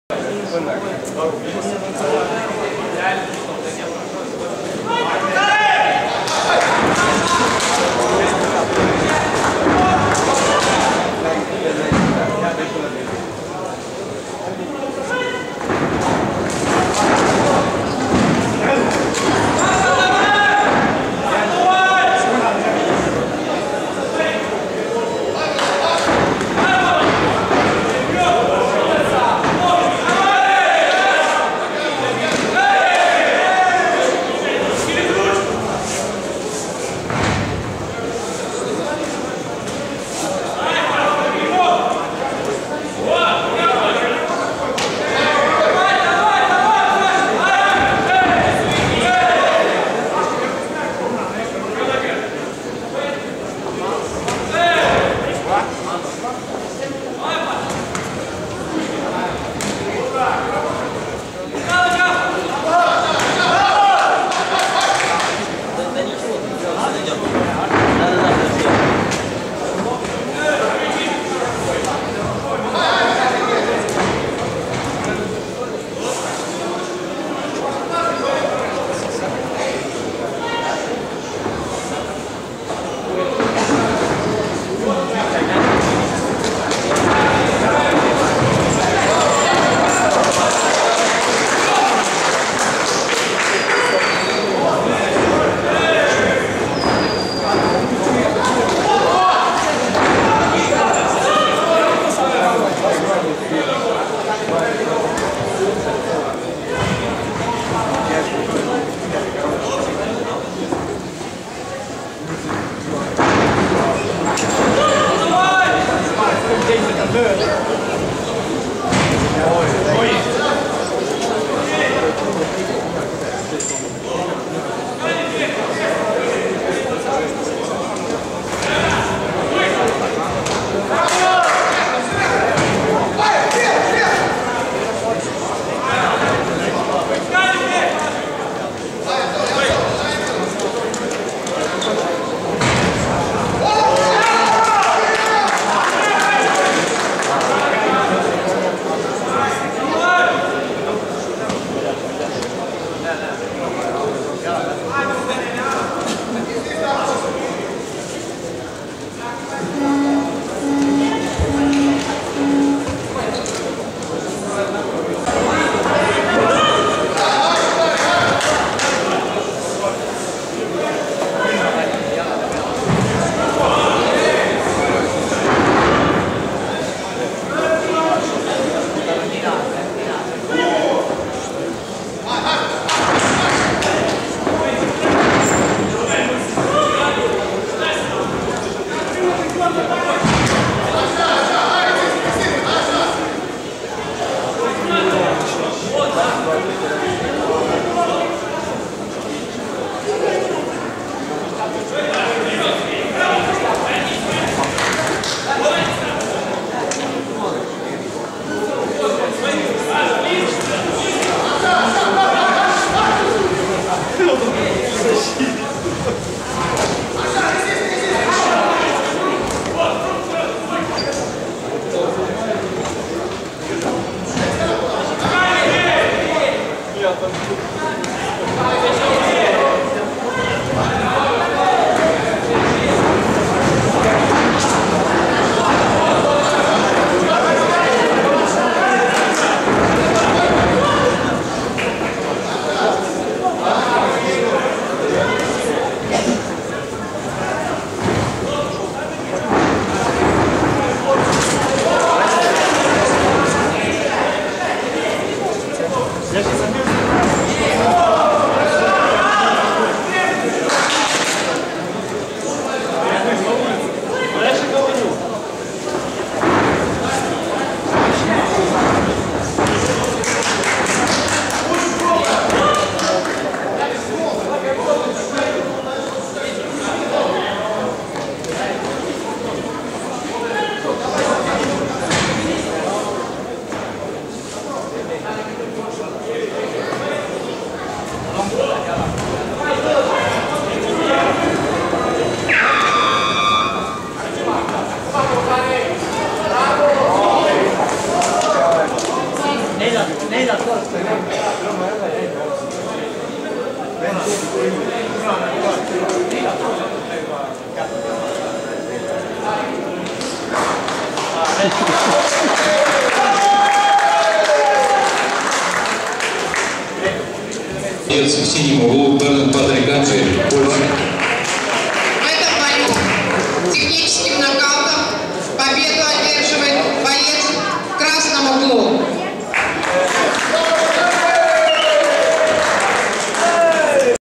Bogdan Stanciu vs Mazurok Pavel-World Cup K-1-Riga 04.10.2013-First Fight.
WAK-1F.
Riga-Letonia.
Bogdan Stanciu from Kudokai Atlassib.